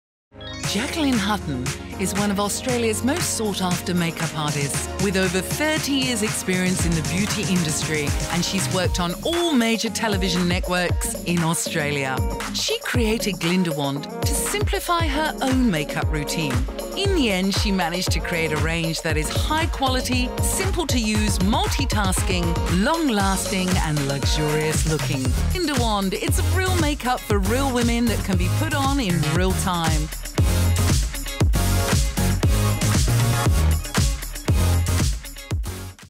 Jacqui Hutton - Founder of GlindaWand
Jacqueline Hutton, the founder of GlindaWand™ and Professional  international celebrity hair and makeup artist.
Jacqui has created a simplified, multi-tasking, luxe looking Makeup Range to make your makeup regime quick, easy and enjoyable again.
"Real Makeup for Real Women in Real Time"
With over 30 years industry experience, Jacqui simplifies and shows you make up applications so you can create the makeup looks you want no matter what your skill level.
Insta;  @GlindaWand
FaceBook;  @GlindaWandbyJacqui